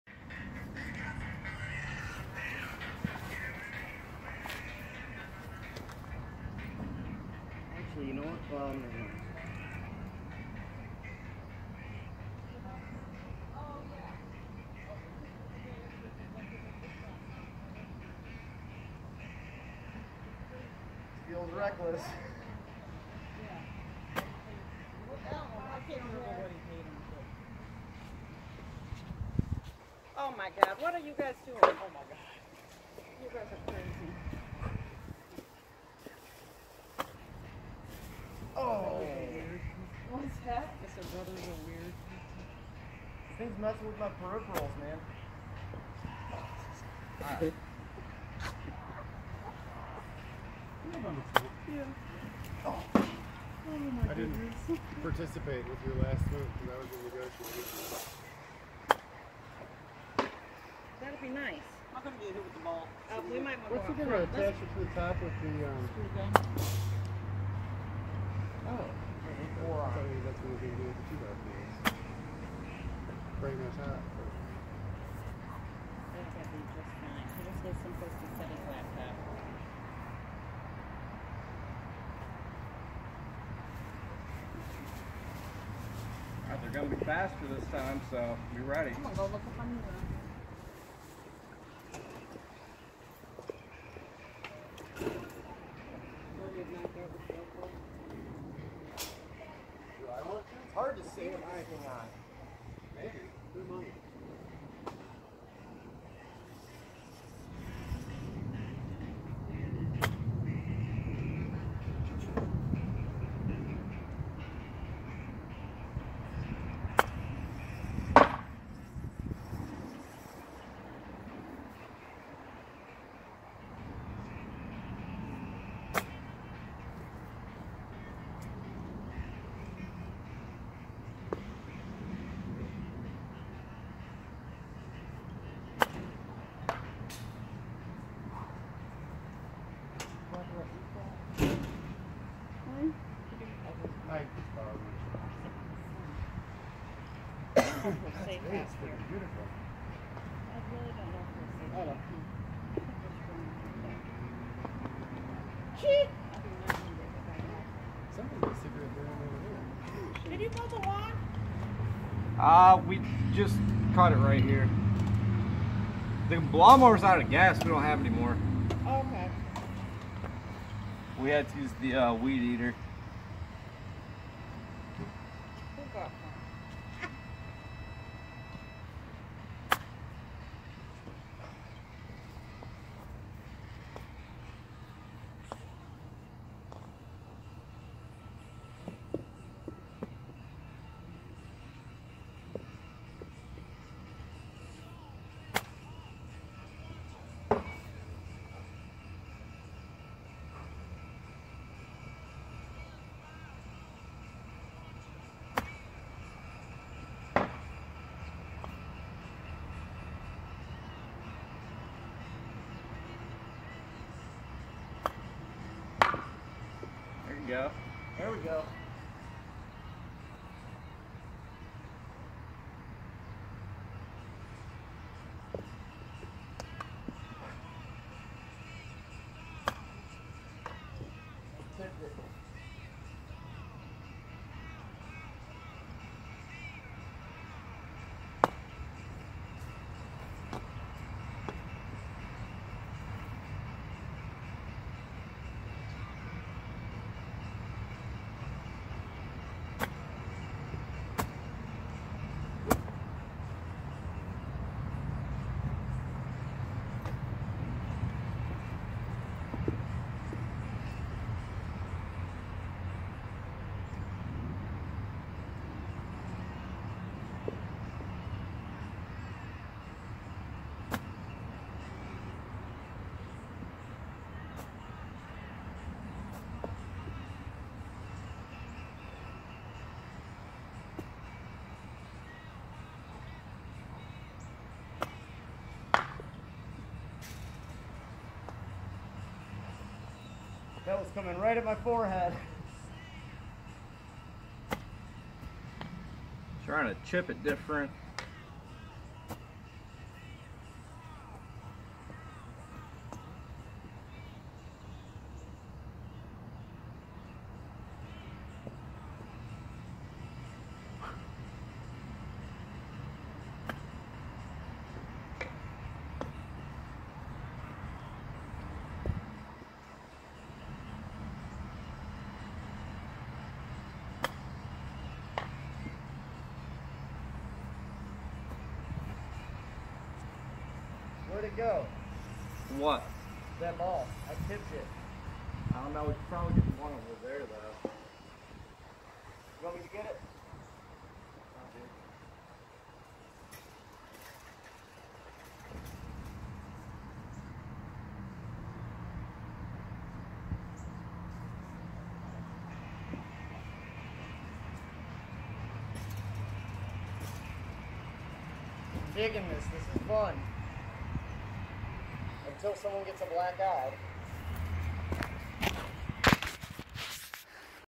Shane's Golf Practice/Ryan's Bo-Staff Defense Practice
When one brother is an amateur golfer, and one brother is a martial arts enthusiast...combine your talents and make a game out of it!